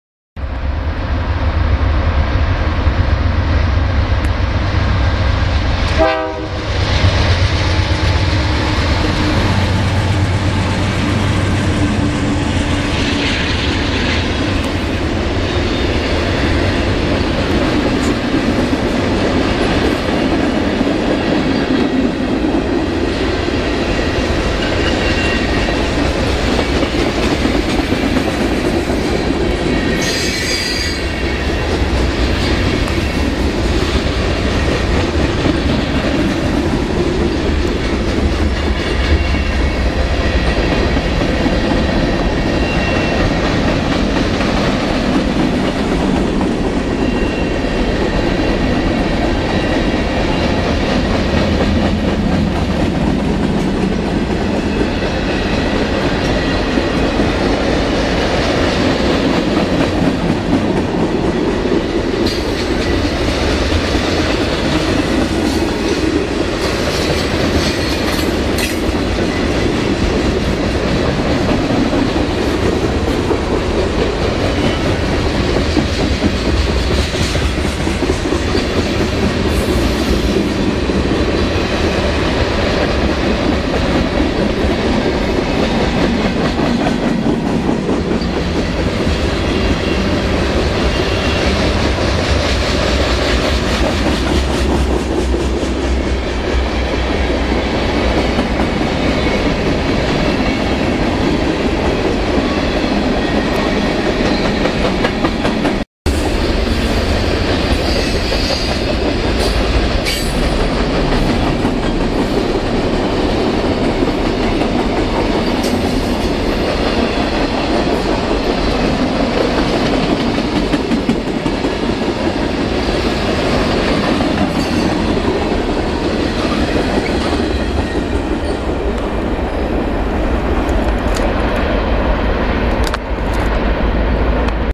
Southbound BNSF Frieght With A Barge in view ALONG The River k line action.
southbound at 8:10am with a another to follow him,